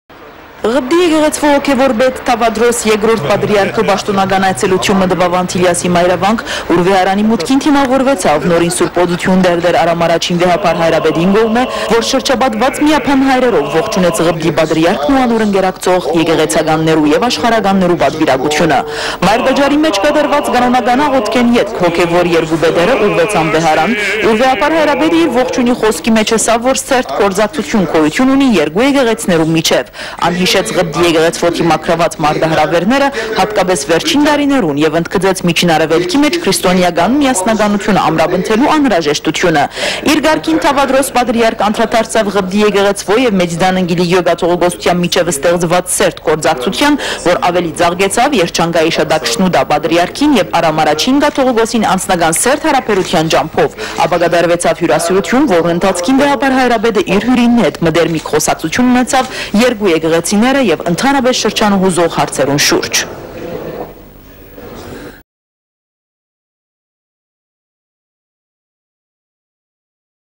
Ղպտի Եկեղեցւոյ Հոգեւոր Պետը Այցելեց Արամ Ա. Կաթողիկոսին
Ղպտի Եկեղեցւոյ Հոգեւոր Պետ Թէոդորոս Բ. Պատրիարքը Այցելեց Արամ Ա. Կաթողիկոսին